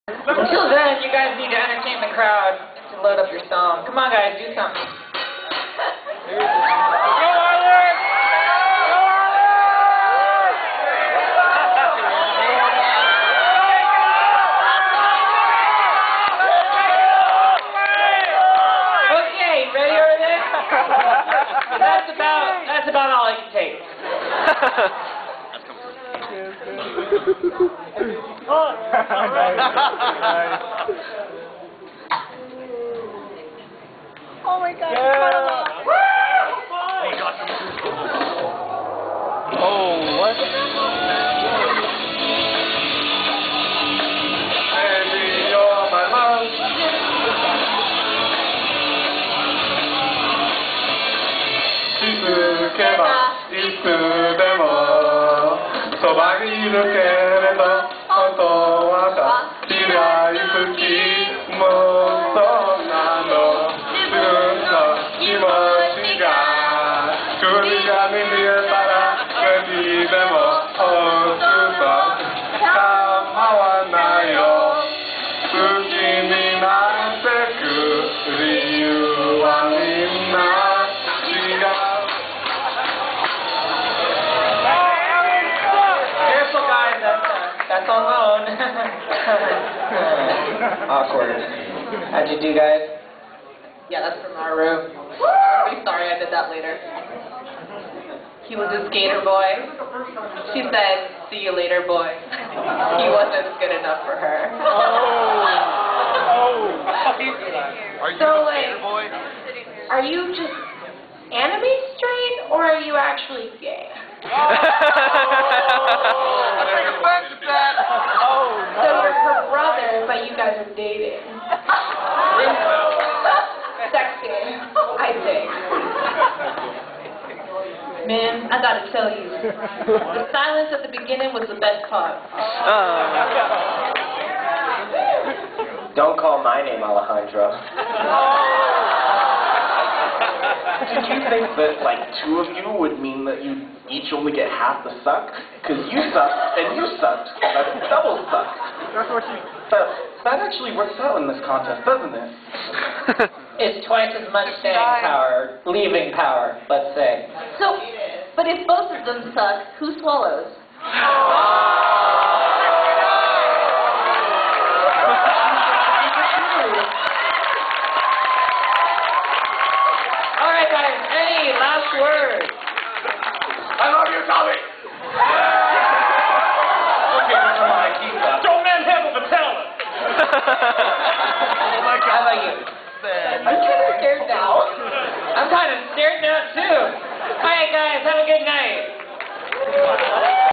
Fanime 2010 Gong Show - Anime Straight Couple
Sorry in advance for bad quality and my stupid laughing. This was a good attempt at bad Japanese and perfect for a Gong Show.